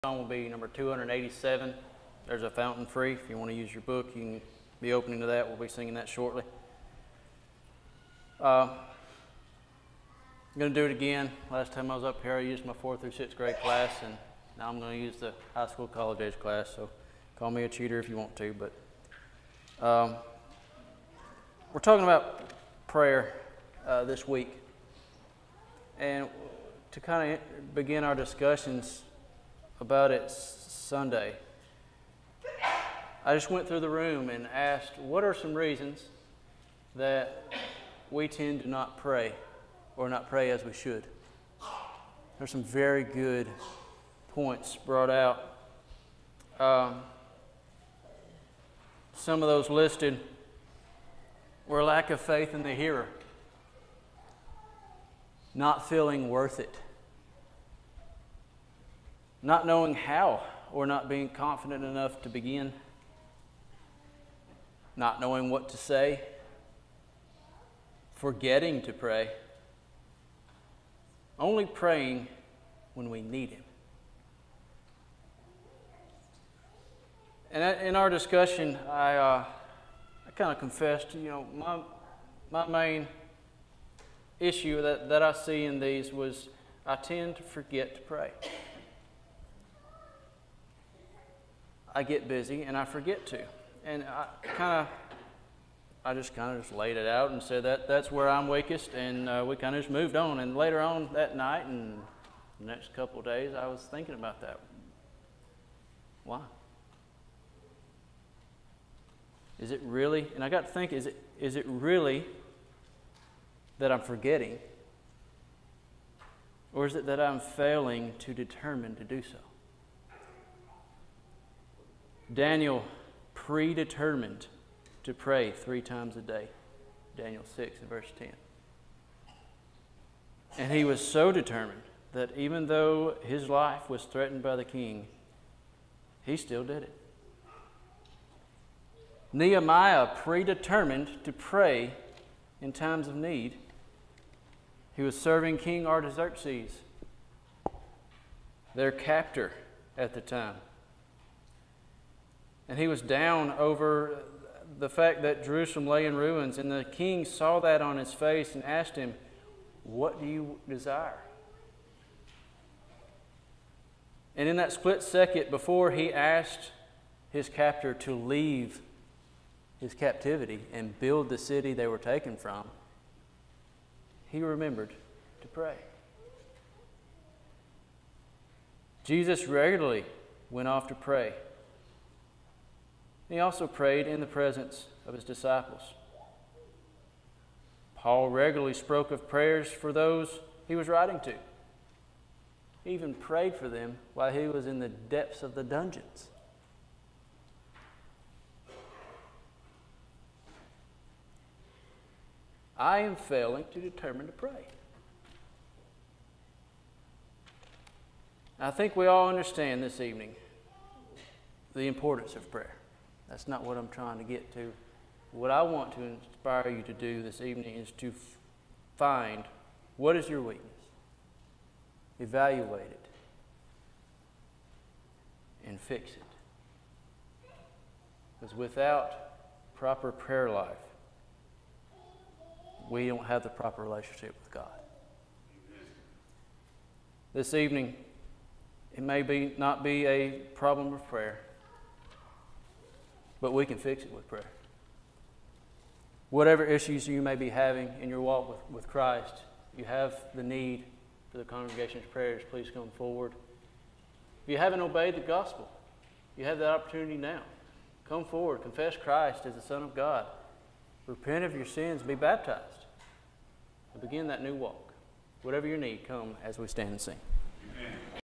Determination in prayer
Speaker: Breck Johnson
Original Recording: 05/01/24

This lesson discusses the importance of determination in one’s prayer life, highlighting common reasons why people may fail to pray and emphasizing the need for a strong prayer life to maintain a proper relationship with God. It encourages viewers to evaluate their weaknesses in prayer and to make a determined effort to address them, drawing inspiration from biblical figures like Daniel, Nehemiah, and Jesus who exemplified a committed prayer life.

00:00:00 Reasons for Not Praying
00:01:58 The Need for Determination in Prayer
00:03:30 Improving Prayer Life